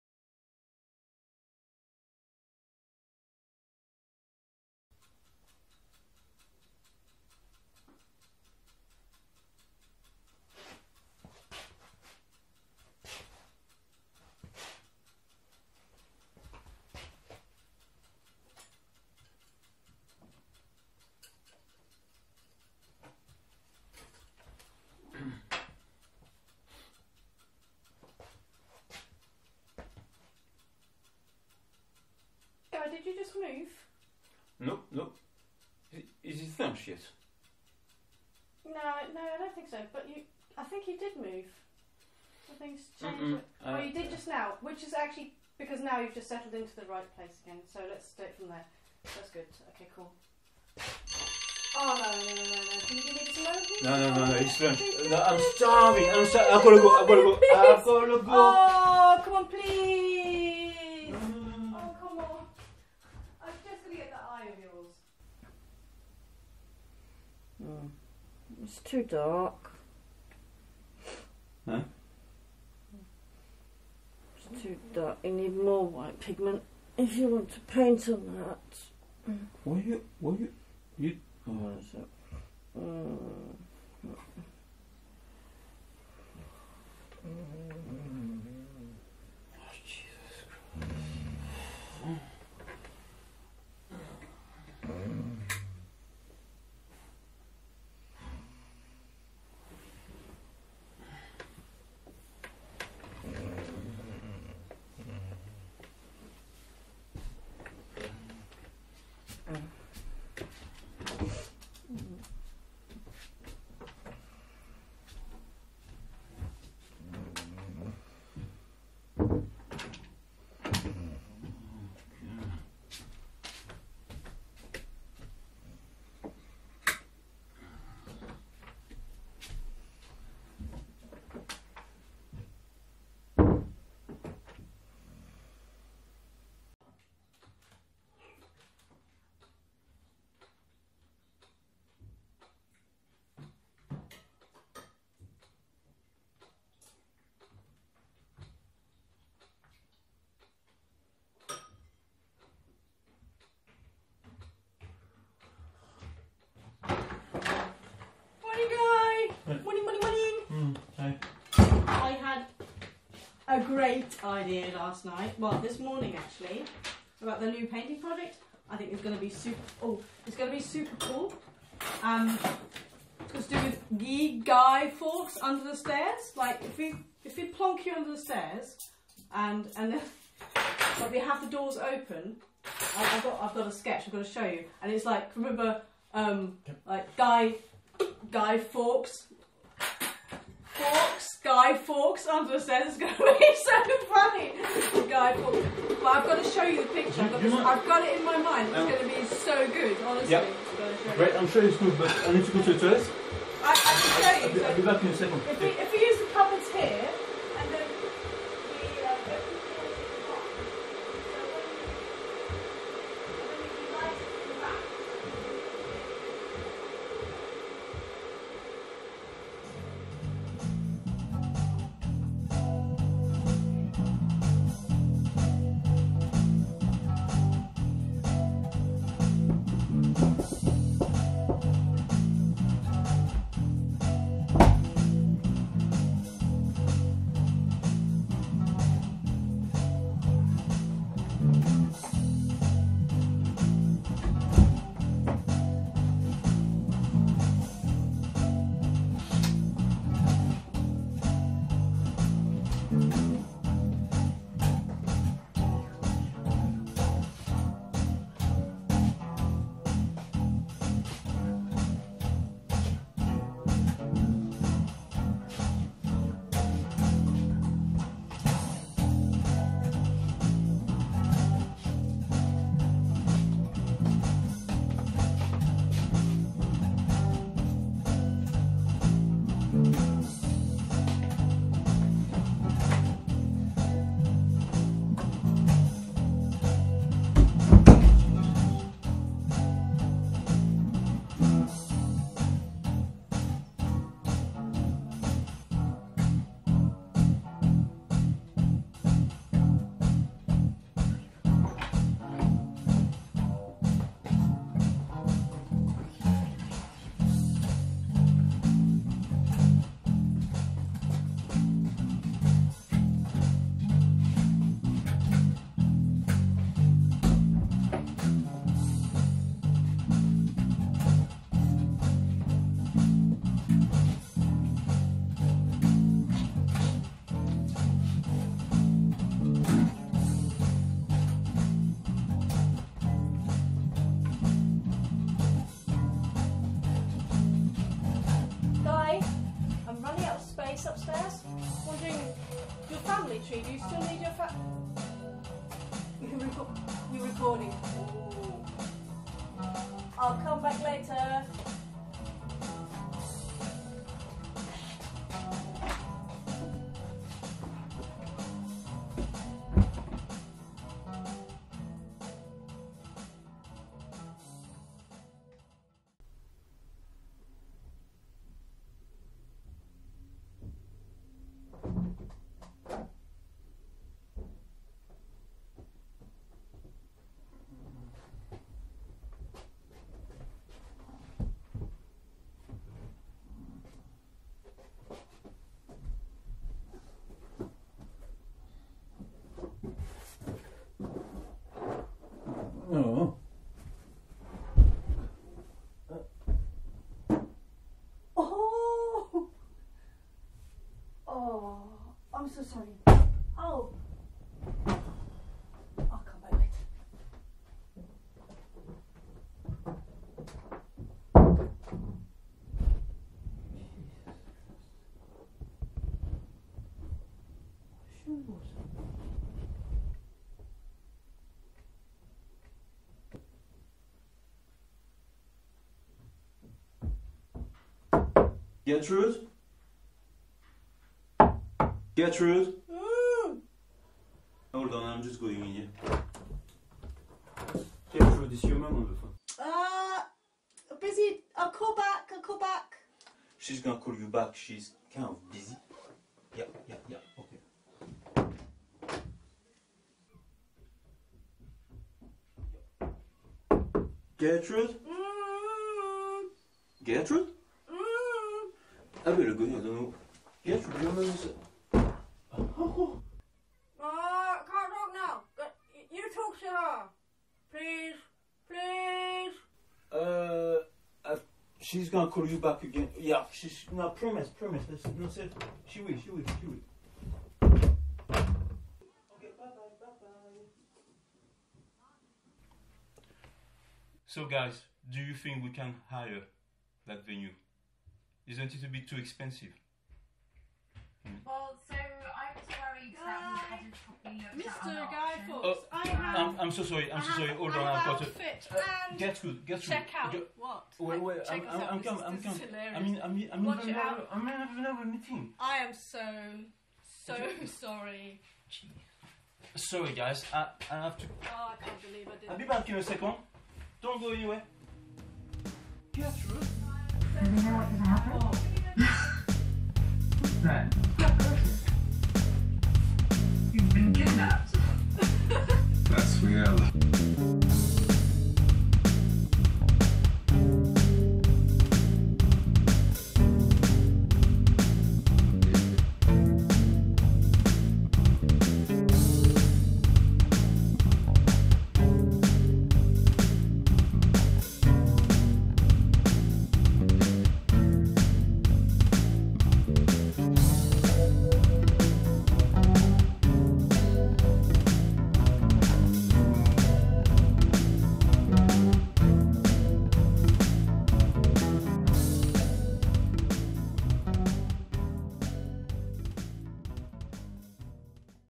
Lockdown with an Artist
Comedy. Some of the days during Covid-19 lockdown